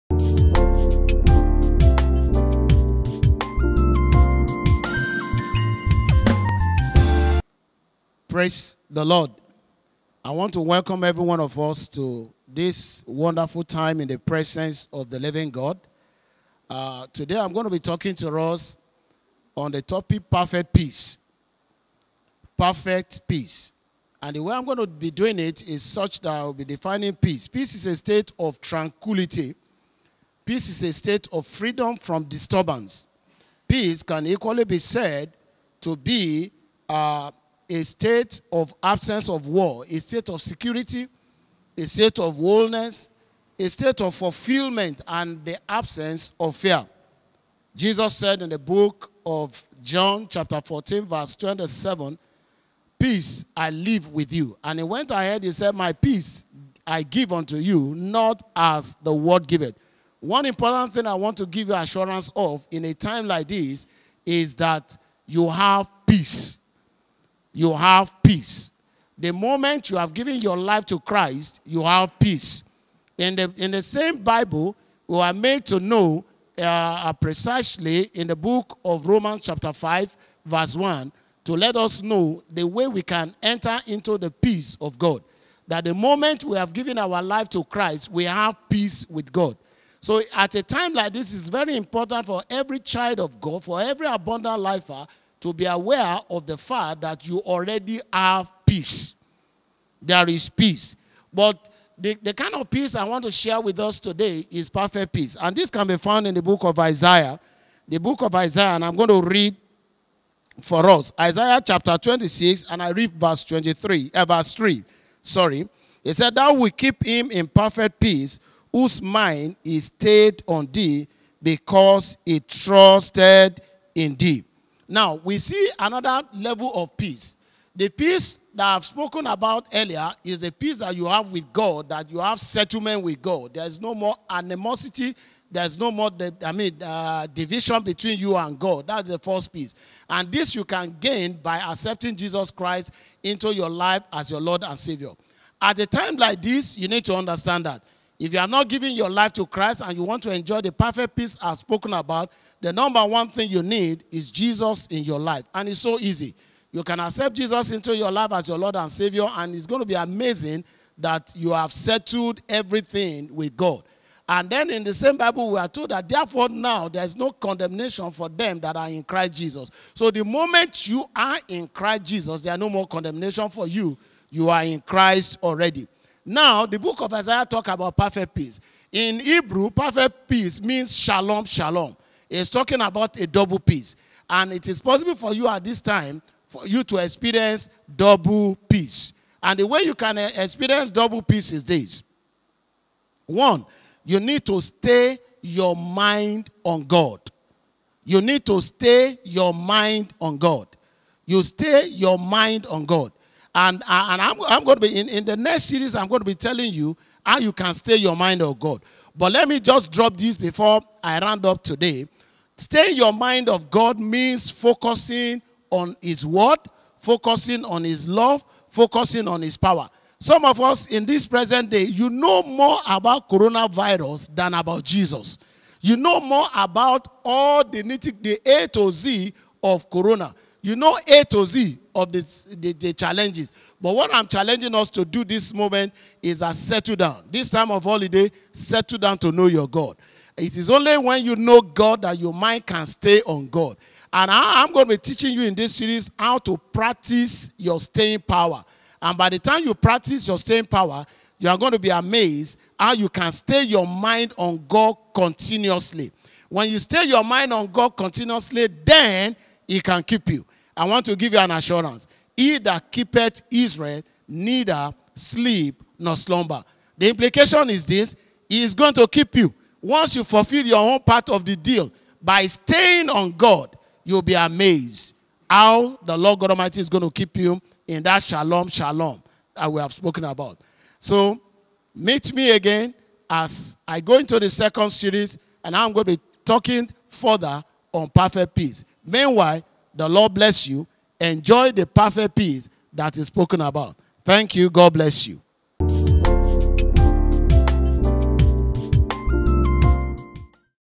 Perfect Peace 1: By Rev Owolabi Of Abundant Life Gospel Church Ibadan
Topic: Perfect Peace part 1
Preacher: Rev Tunde Owolabi (State Pastor)
Church: Abundant Life Gospel Church Ibadan